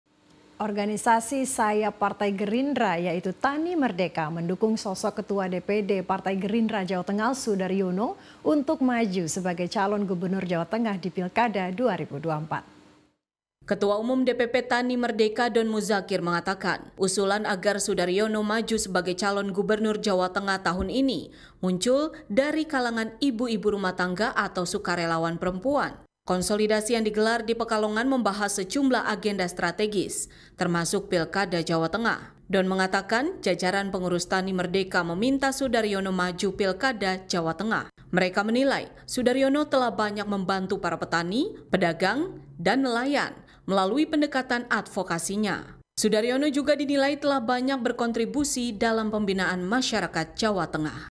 Sudaryono Dapat Banyak Dukungan untuk Jadi Calon Gubernur Jawa Tengah
Organisasi sayap Partai Gerindra, yaitu Tani Merdeka, mendukung sosok Ketua DPD Partai Gerindra Jawa Tengah, Sudaryono, untuk maju sebagai calon Gubernur Jawa Tengah di Pilkada 2024.